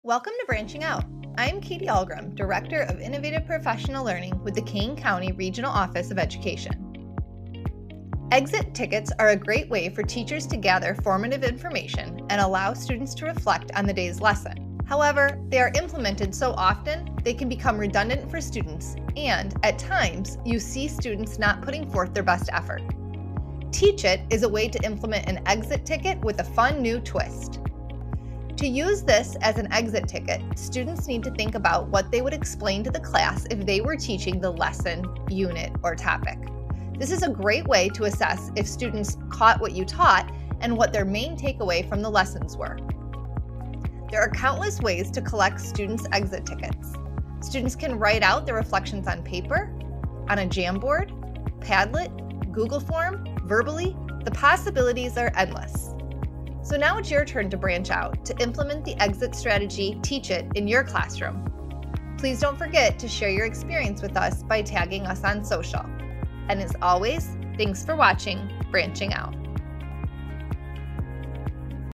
Branching Out: Teach It
Exit tickets are a great way for teachers to gather formative information and allow students to reflect on the day’s lesson; however, they are implemented so often they can become redundant for students and at times, you see students not putting forth their best effort.  “Teach It” is a way to implement an exit ticket with a fun new twist!